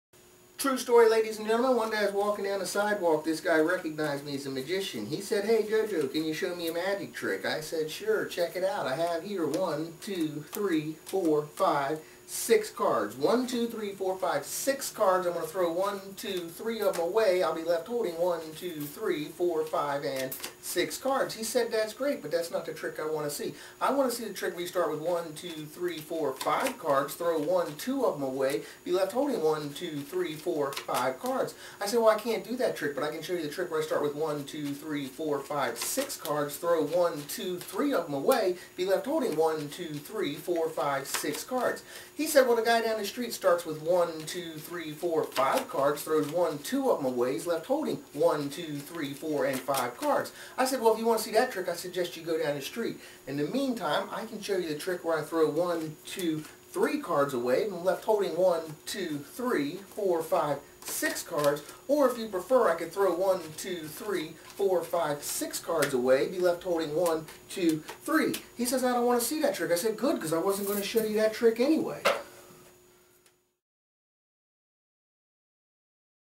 Six Card Repeat
New routine I'm working on ... might make the rotation at Barefoot Landing this summer. :)